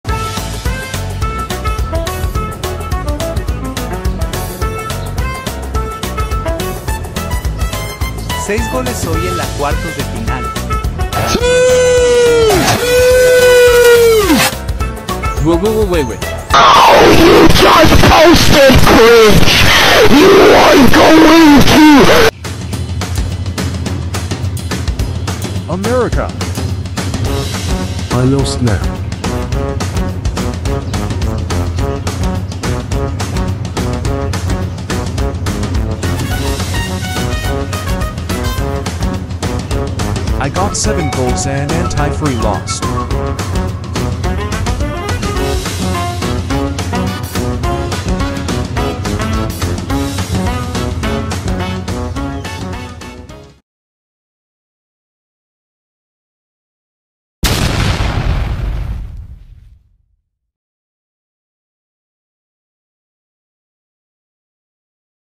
FIFA World Cup Republic Of California 2023 Quarter Finals Cuartos De Final
I made a Quarter Finals World Cup concept video guys the video made by me ​⁠@TheStarWhoLikesChocolate_AAN ​⁠@JedDrawStuff ​⁠@Crowny.